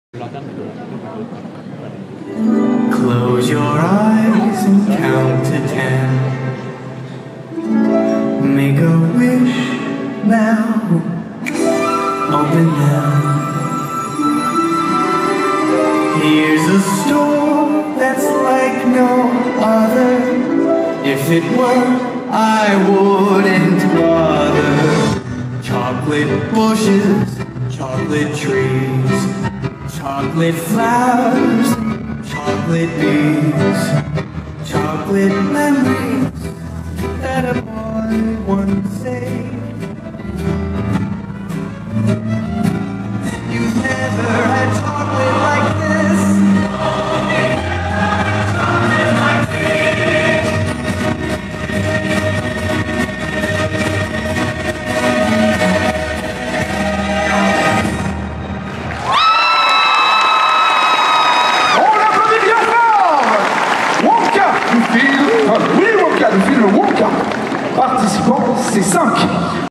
Japan Expo 2024 - Cosplay Vendredi - 13 - Willy Wonka
Cosplay Credit: (space reserved for the cosplayer credit, if you are on the video and you wish for a credit, feel free to comment or contact me to give me the appropriate information to add.
(Emplacement réservé pour le crédit des cosplayeurs... si vous désirez un crédit, vous pouvez m'indiquer lequel mettre, s'il y a un souci quelconque vous me contactez...)

Cosplay Contest organized during the Friday of Japan Expo in July 2024
at The Park des Exposition de Villepinte
Concours Cosplay organisé pendant le vendredi de Japan Expo en juillet 2024
au parc des Exposition de Villepinte

Remerciements divers:
* à l'équipe du festival.
* à l'association gérant les concours.
* aux cosplayeurs.

-- Various links